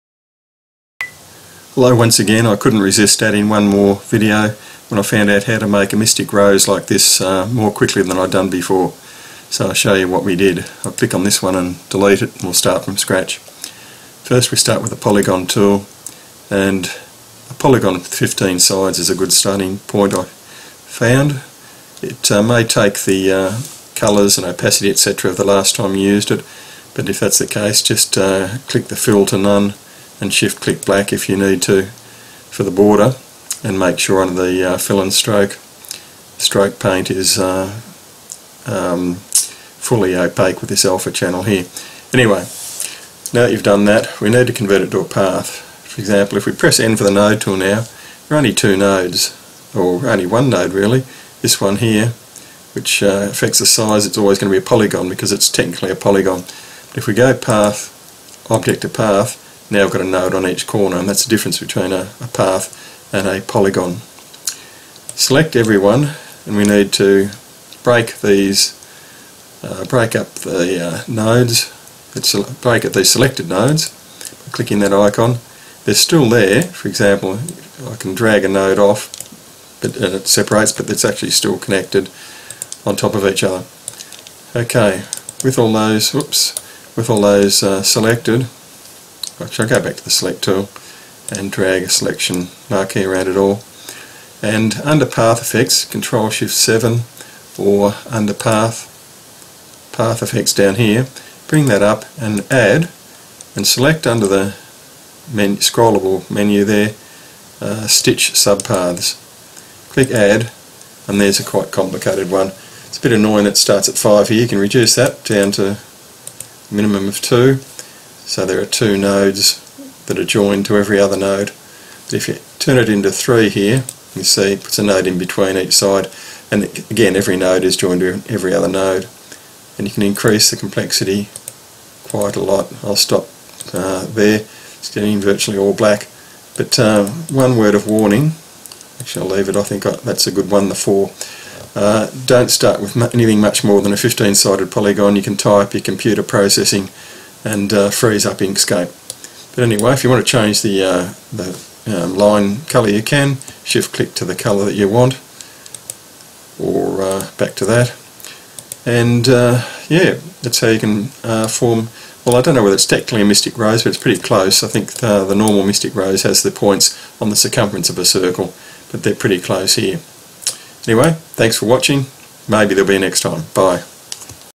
Quick Mystic
How to produce a Mystic Rose pattern in just a few keystrokes using Inkscape's Path effects editor.